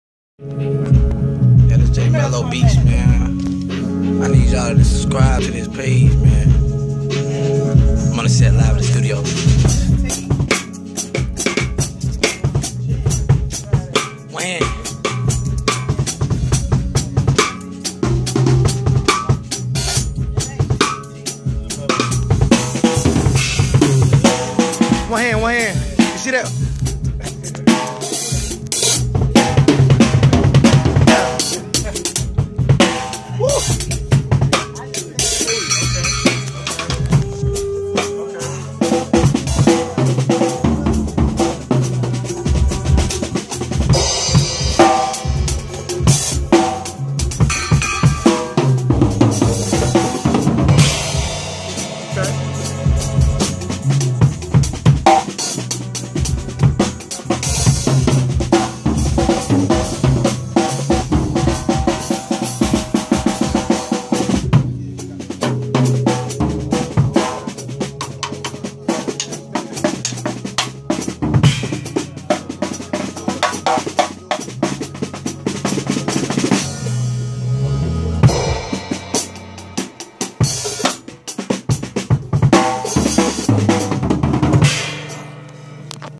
J-Melo Beatz @Studio LA in Charlotte on the drumset wit one stick
One stick drumming